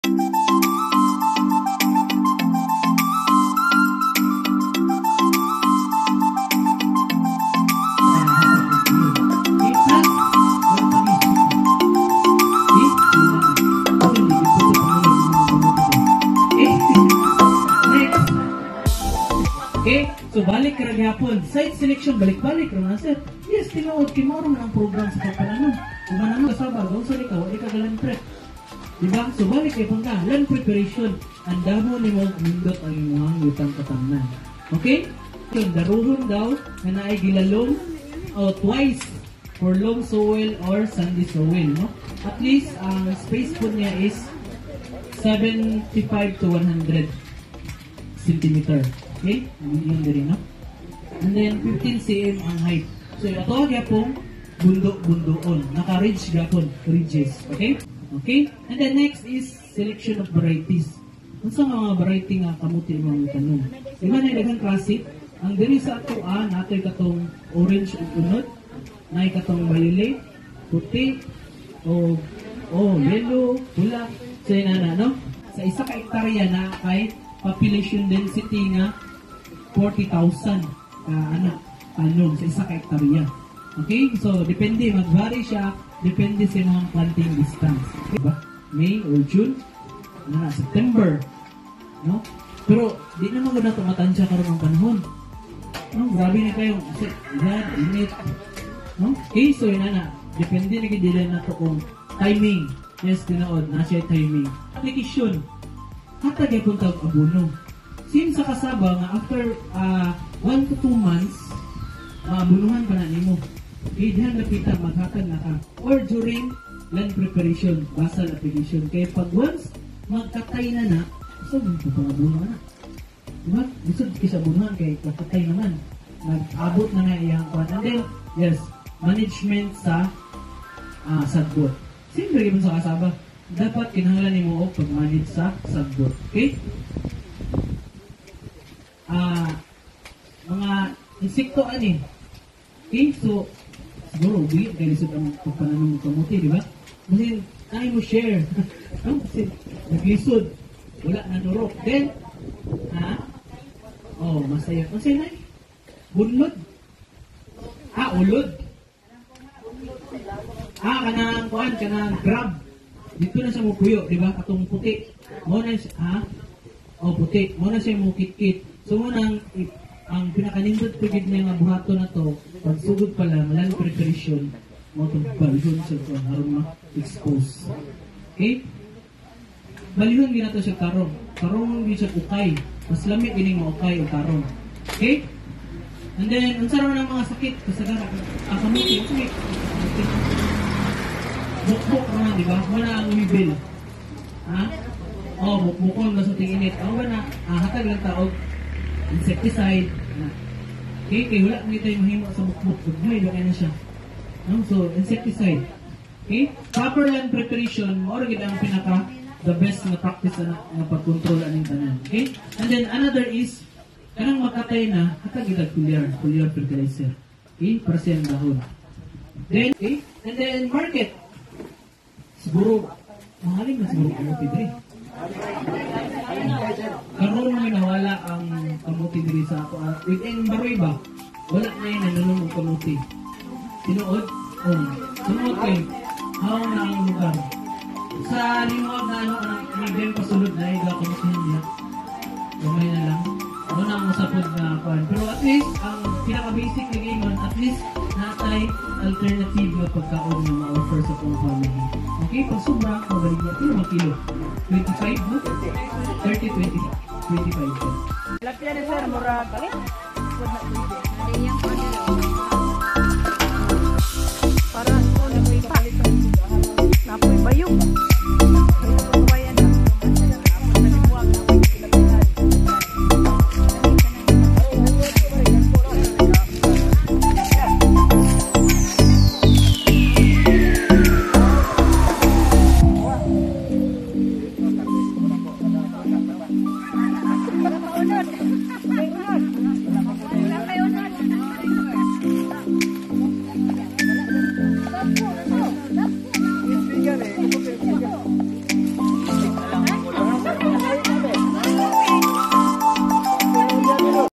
Blando’s Farm Episode 21: How to Plant Sweet Potatoes or Kamote.
The technical team from DA (Dept of Agriculture) Lanao del Norte held a Training Session at Blando’s Farm in Riverside, Baroy, Lanao del Norte August 30, 2023 about how to plant this root crop called Kamote.